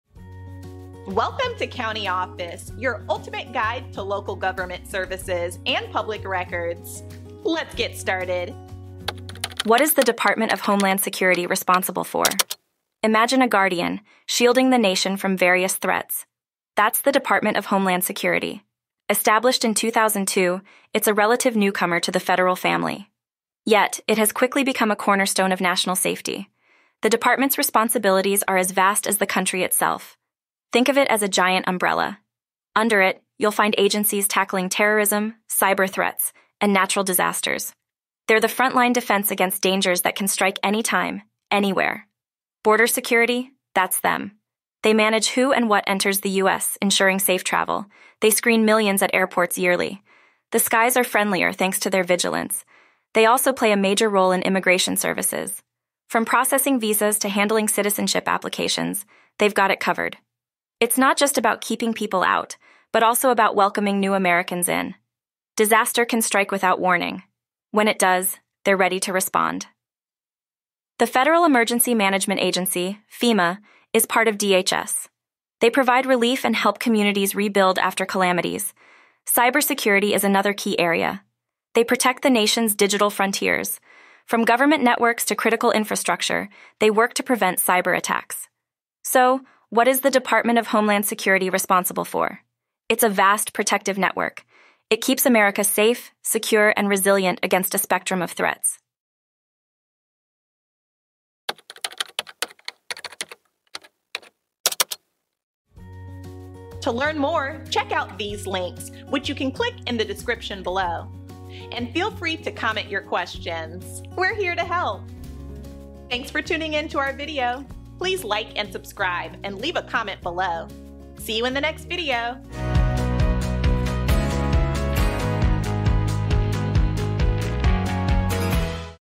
What Is The Department Of Homeland Security Responsible For? - CountyOffice.org
What Is The Department Of Homeland Security Responsible For? Discover the multifaceted role of the Department of Homeland Security (DHS), the federal agency that stands as a guardian of the United States. Since its inception in 2002, DHS has become an integral part of the nation's security framework, addressing a wide array of threats to keep America safe and resilient. From border security to disaster response, this video delves into the critical functions and agencies that operate under the DHS umbrella. Learn how DHS manages border control, immigration services, cybersecurity, and emergency management through its various agencies, including the Federal Emergency Management Agency (FEMA). Join us as we explore the vast protective network that is the DHS, and understand the significance of its mission to safeguard the nation against diverse dangers.